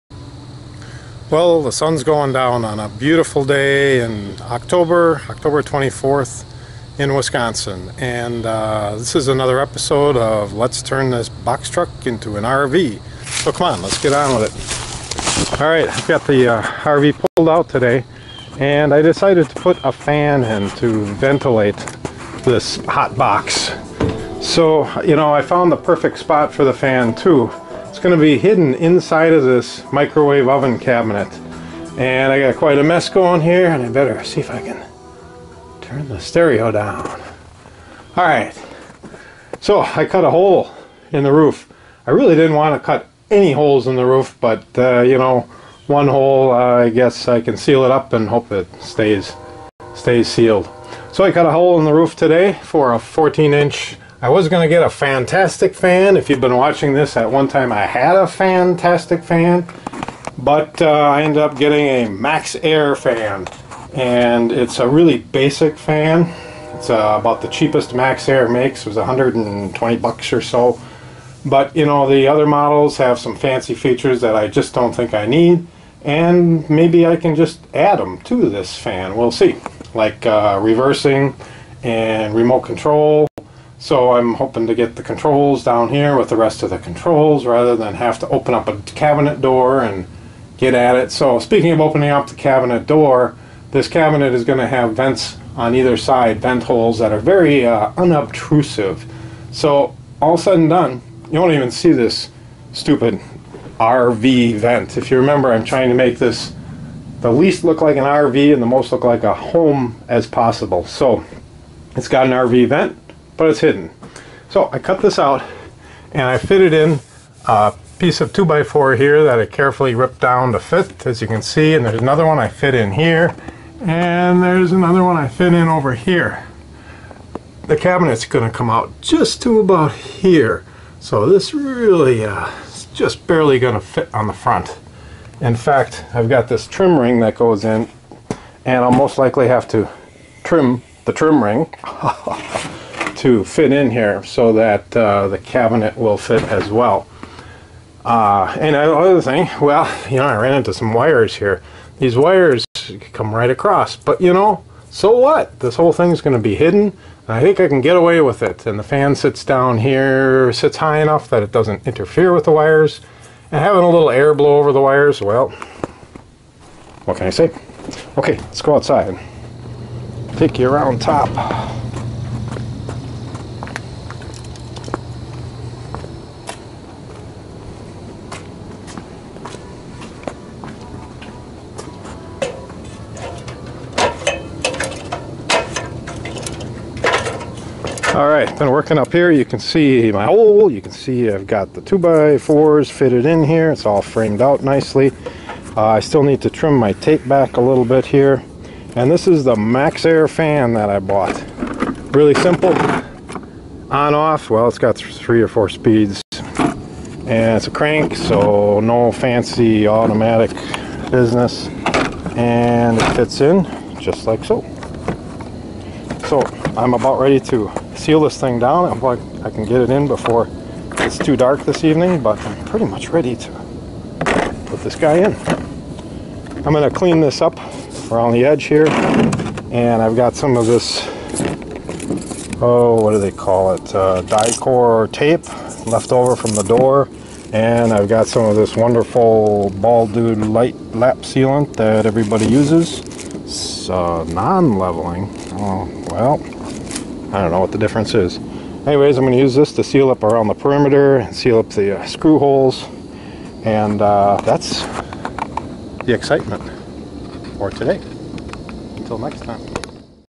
Rich's Box Truck to RV #80 - MAXXAIR ceiling fan rough installation
Rich decides in install a fan above the microwave oven cabinet.